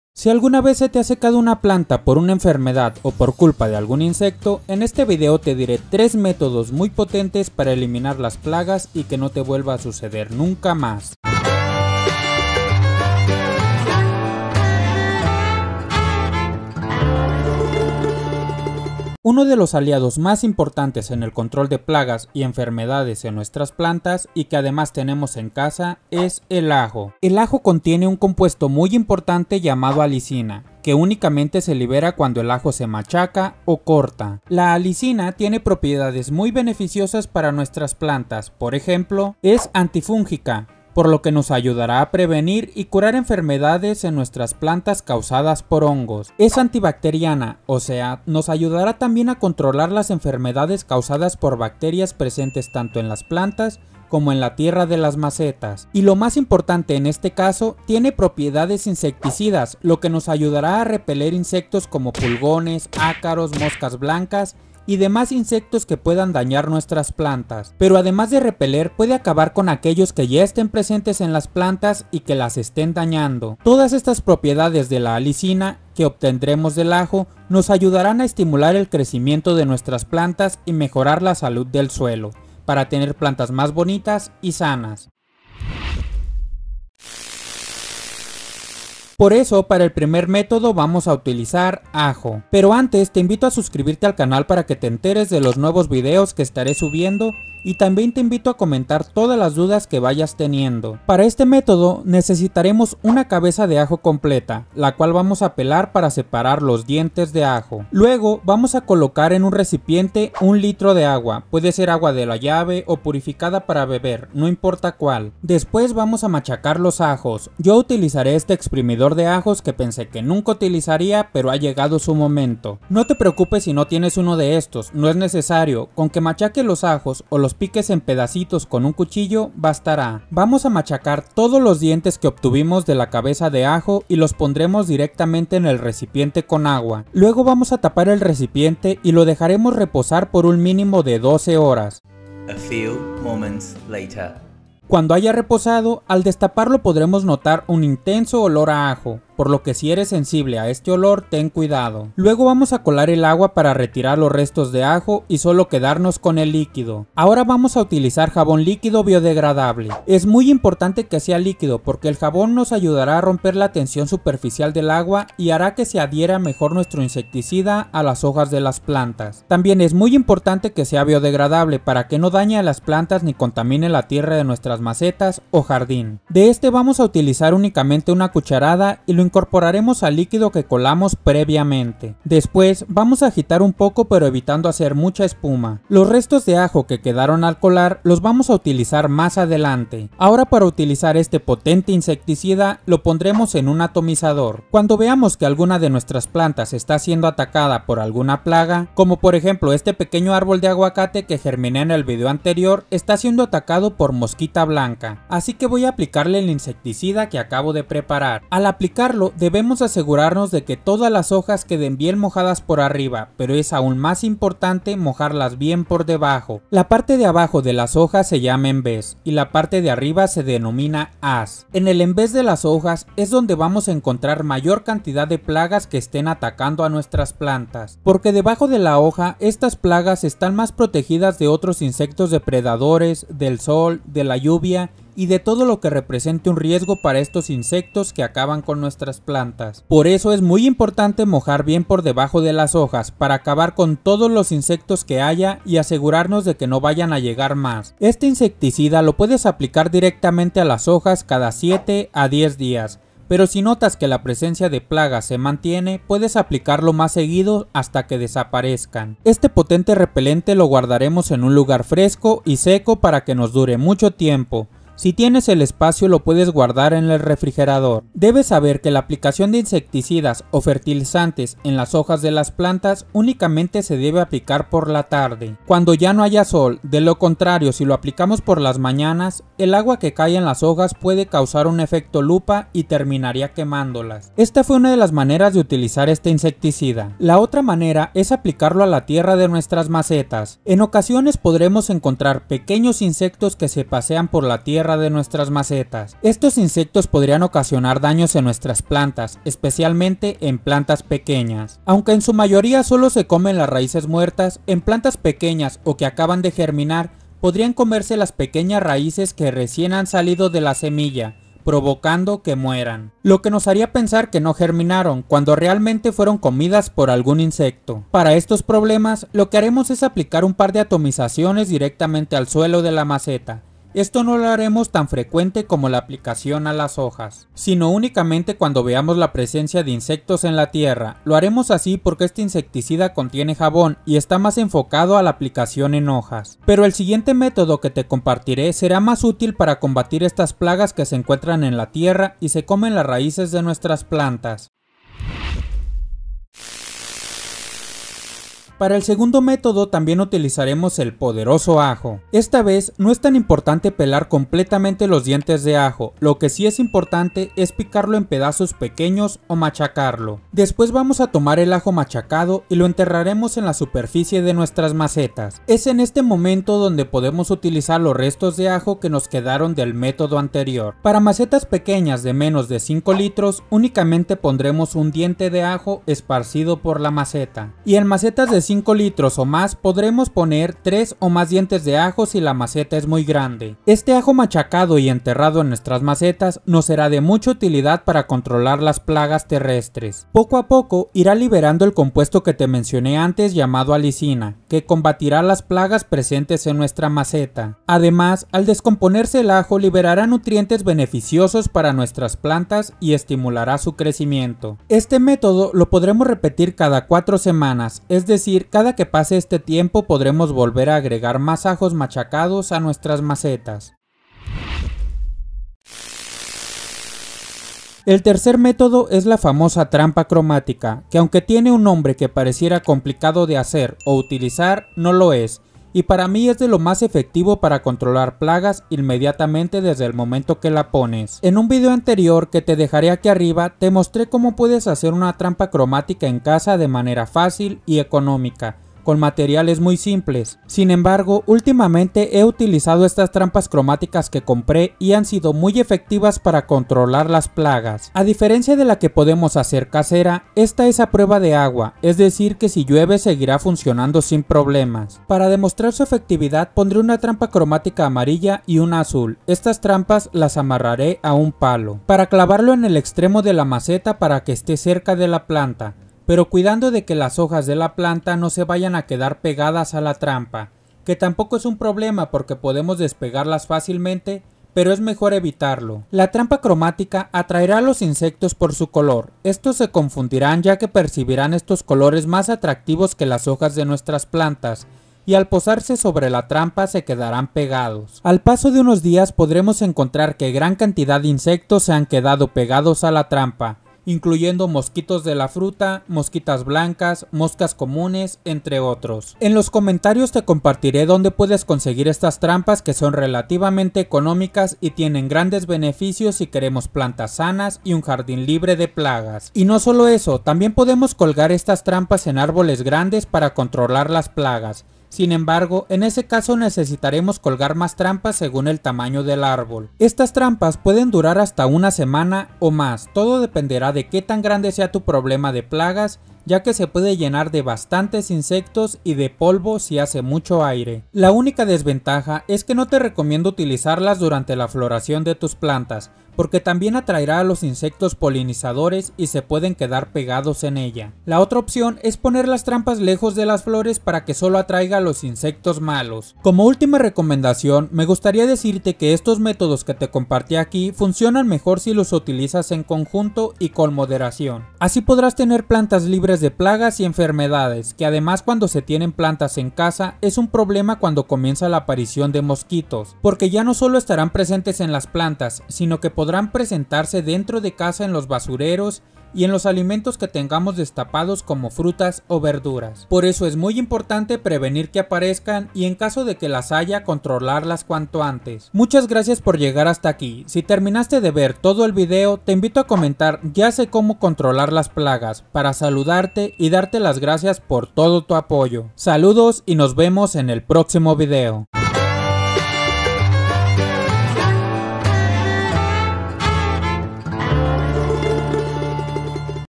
🦗3 INSECTICIDAS CASEROS Potentes para ELIMINAR PLAGAS de tus Plantas Rápidamente 🌿
👨‍🌾 Hola horticultores y horticultoras! 👩‍🌾
En este vídeo te mostrare como puedes hacer 2 insecticidas caseros para controlar, eliminar y prevenir que aparezcan las plagas en tus plantas de una manera muy fácil. También te mostrare una trampa, llamada trampa cromática, que es muy efectiva para proteger tus plagas de las trampas.

Si alguna vez se te ha secado una planta por culpa de una enfermedad o un insecto, en este vídeo te diré 3 métodos muy potentes para eliminar las plagas y que no te vuelva a suceder nunca más.
AJO CON JABÓN
Introducción
Uno de los aliados más importantes en el control de plagas y enfermedades en nuestras plantas y que además tenemos en casa, es el ajo.
El ajo contiene un compuesto muy importante llamado alicina, que únicamente se libera cuando el ajo se machaca o corta.
La alicina tiene propiedades muy beneficiosas para nuestras plantas, por ejemplo:
Es antifúngica, por lo que nos ayudará no solo a controlar plagas sino también a prevenir y curar enfermedades en nuestras plantas causadas por hongos.
Es antibacteriana, o sea, nos ayudará también a controlar las enfermedades causadas por bacterias en las hojas y en la tierra de nuestras plantas.
Y lo más importante en este caso, tiene propiedades insecticidas, lo que nos ayudará a repeler insectos como pulgones, ácaros, moscas blancas y demás insectos que puedan dañar nuestras plantas, pero además de repeler puede acabar con aquellos que ya estén presentes en las plantas y que las estén dañando.
Todas estas propiedades de la alicina que obtendremos del ajo, nos ayudarán a estimular el crecimiento de nuestras plantas y mejorar la salud del suelo, para tener plantas más bonitas y sanas.
Por eso para el primer método vamos a utilizar ajo.
Pero antes, te invito a suscribirte al canal para que te enteres de los nuevos vídeos que estaré subiendo y también te invito a comentar todas las dudas que vayas teniendo.

⏱⏱⏱ TIMESTAMPS ⏱⏱⏱
00:00 Esto aprenderemos
00:11 Intro
00:19 ¿Porque el ajo funciona como insecticida?
01:25 Método 1: Insecticida con agua, ajo y jabón
01:52 Método 1: ¿Cómo preparar insecticida casero con Ajo?
03:10 Método 1: ¿Cómo utilizarlo?
04:07 Método 1: ¿Cuándo se utiliza?
04:44 Método 1: Otra manera de utilizarlo para plagas terrestres
05:44 Método 2: ¿Para que sirve enterrar un ajo picado?
06:10 Método 2: ¿Cuánto ajo utilizar?
06:53 Método 3: El color amarillo y azul atrae a las plagas
07:35 Método 3: ¿Cómo utilizar una trampa para plagas?
08:51 Método 3: Cuidado con esto
09:06 Esto es muy importante para eliminar las plagas